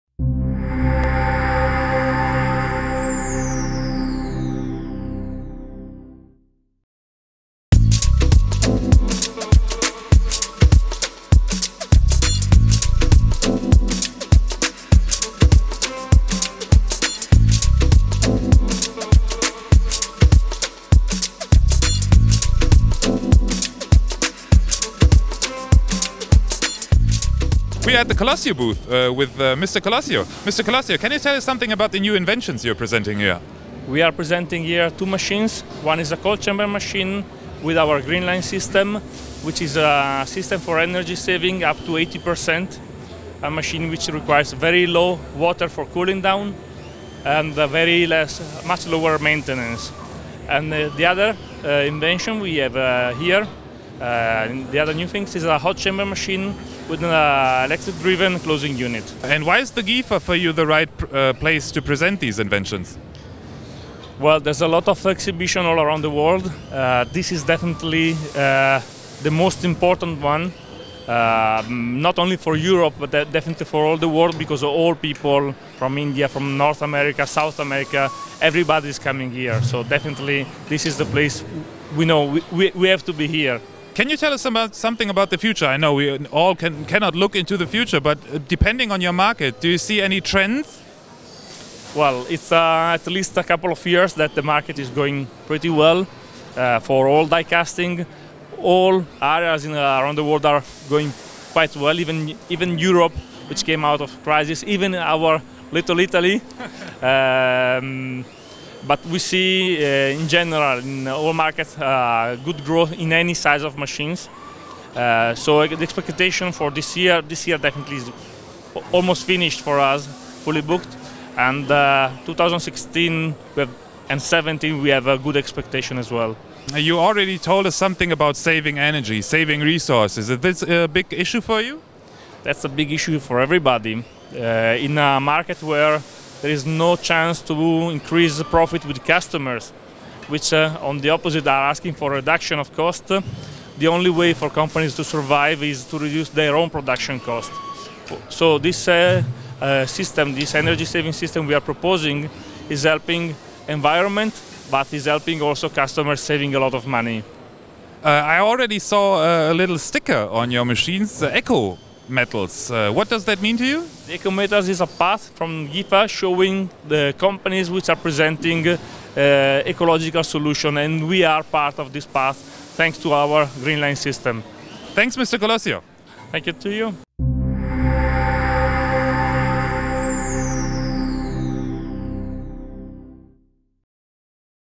GIFA 2015: COLOSIO and two innovations in diecasting
The company COLOSIO is part of the ecoMetals trails and has therefore innovations to exhibit that help saving energy. General Manager Davide Colosio speaks about these machines and is also giving an insight into his future expectations.